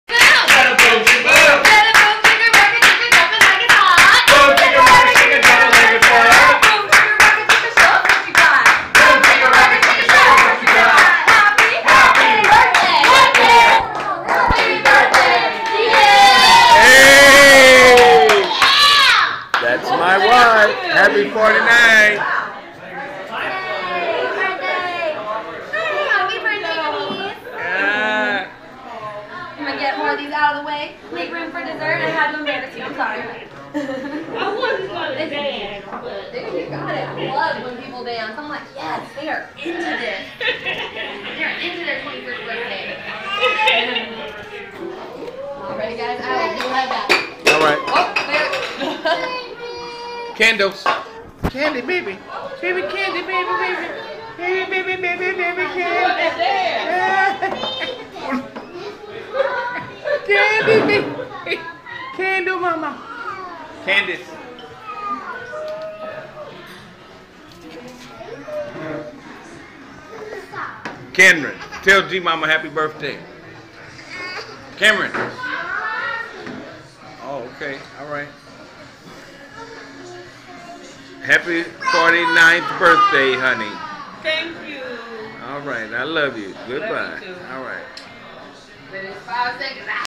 Happy 49th birthday, to you G-Momma...
We took my wife out to Bubba Gump Shrimp, in Long Beach and we had a ball...